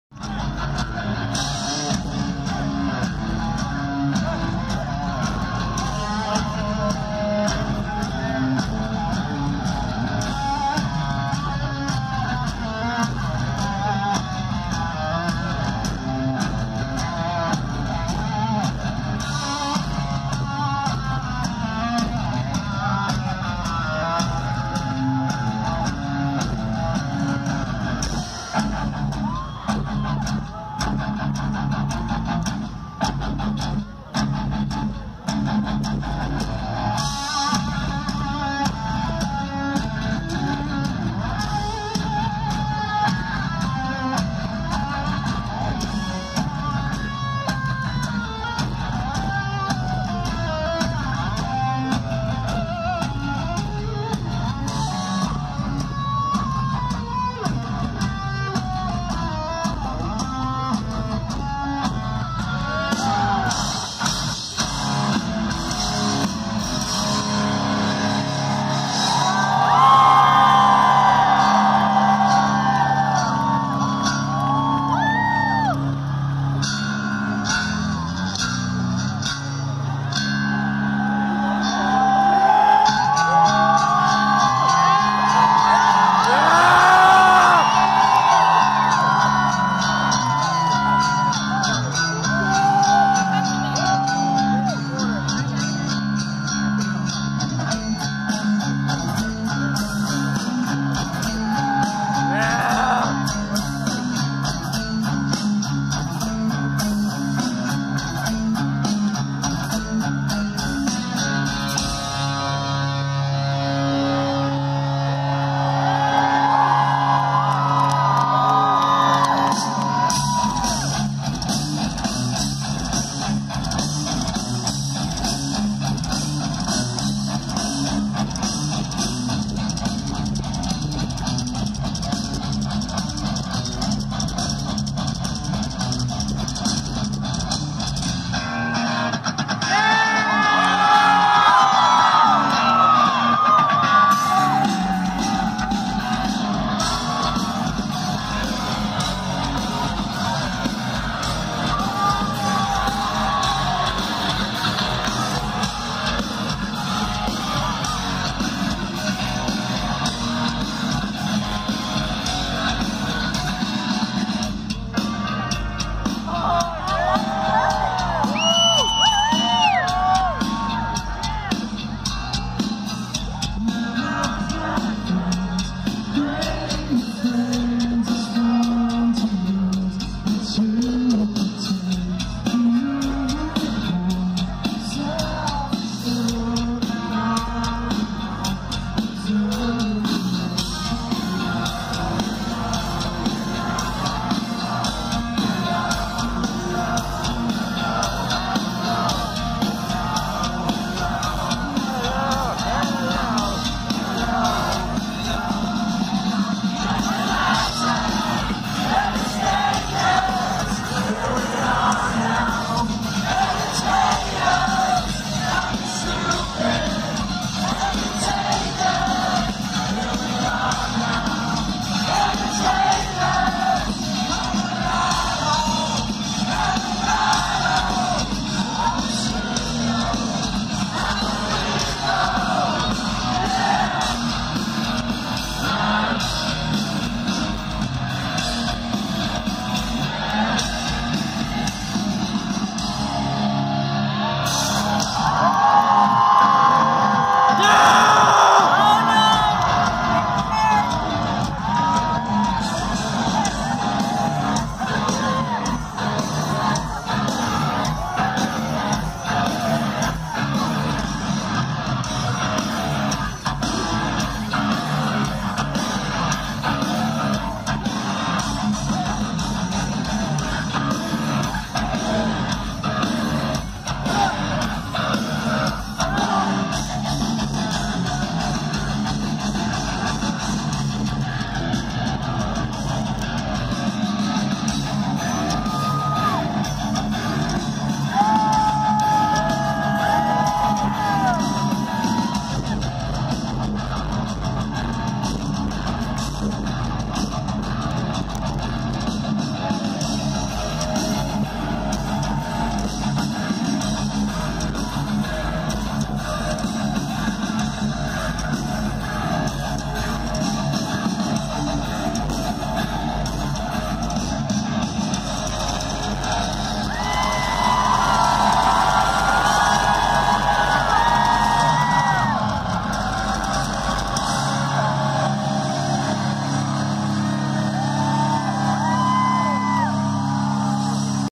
Breaking benjamin at Rockville covers medley
Breaking benjamin covers tool Nirvana and Pantera at Rockville 2018